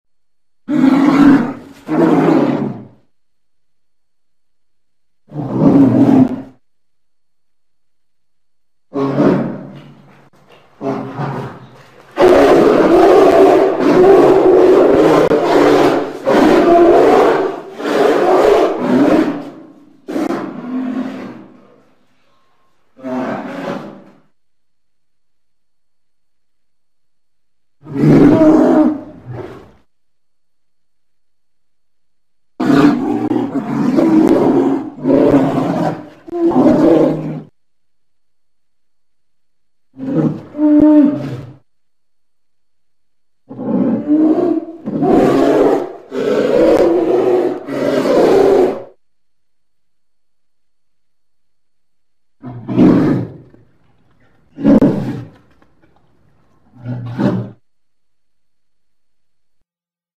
Sound Effects - Polar Bear
The polar bear (Ursus maritimus) is a hypercarnivorous bear whose native range lies largely within the Arctic Circle, encompassing the Arctic Ocean, its surrounding seas and surrounding land masses. It is a large bear, approximately the same size as the omnivorous Kodiak bear (Ursus arctos middendorffi).